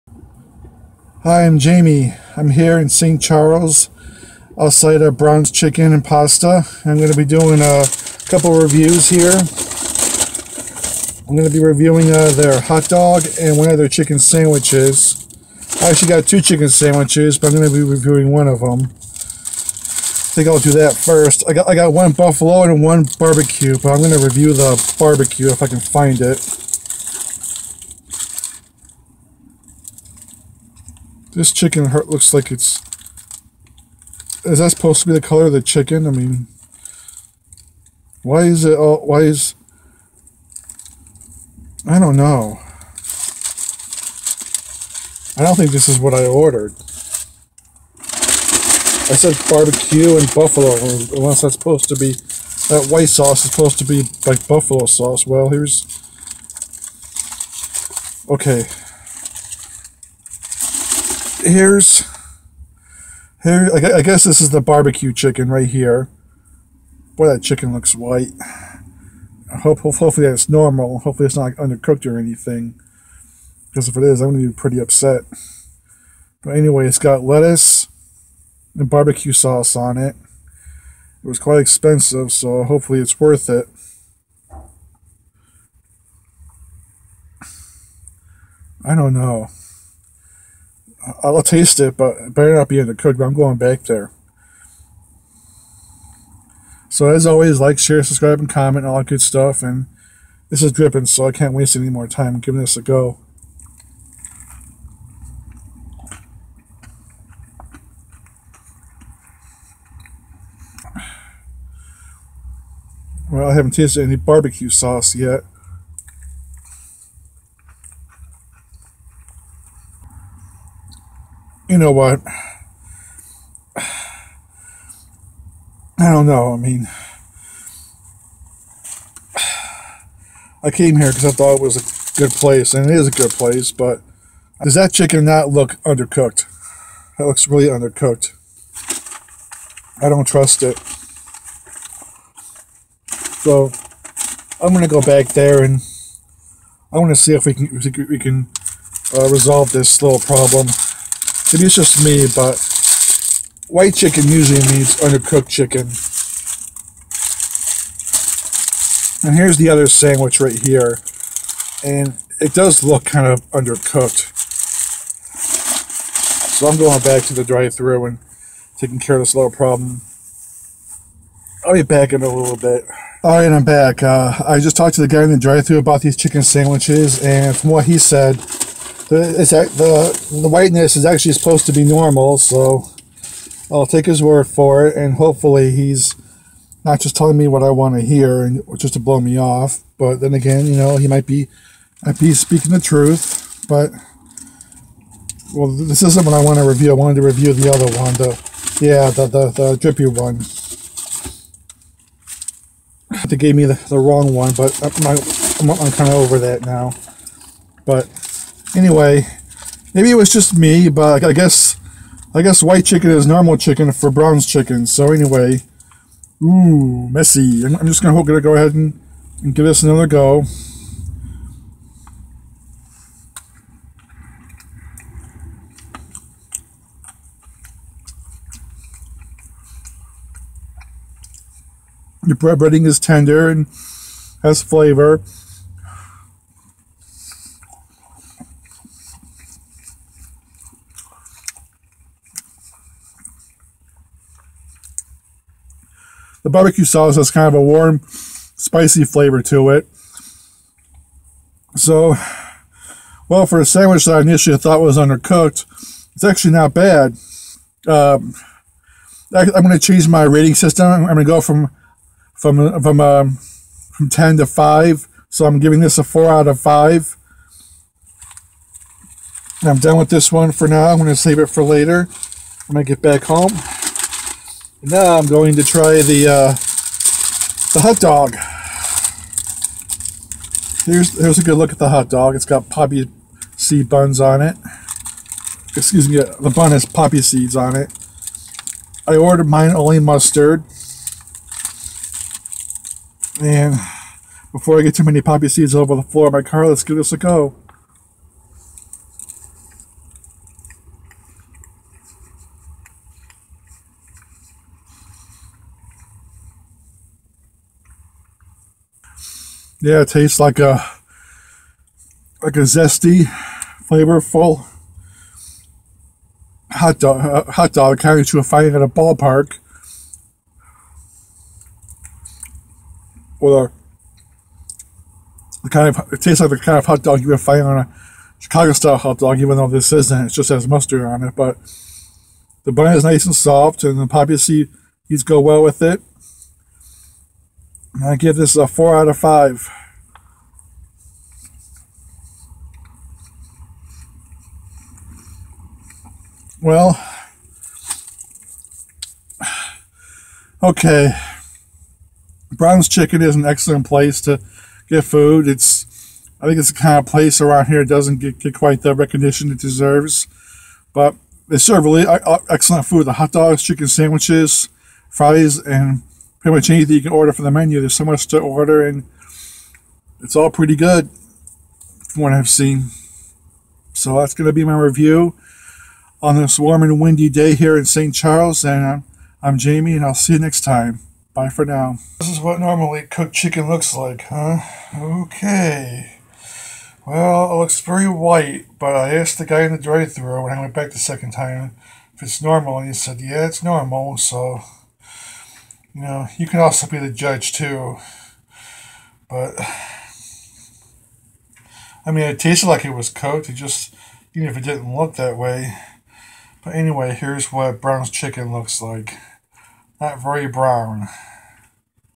Jamie's Product Reviews/Brown's Chicken & Pasta - BBQ Chicken Sandwich & Hot Dog
The chicken patties on both my chicken sandwiches were kinda white which made me suspect that the chicken wasn't fully cooked.  After going through the drive-thru a second time, an employee claimed the whiteness was normal for their chicken.  I still don't know.  But for potentially undercooked chicken, at least it tasted decent...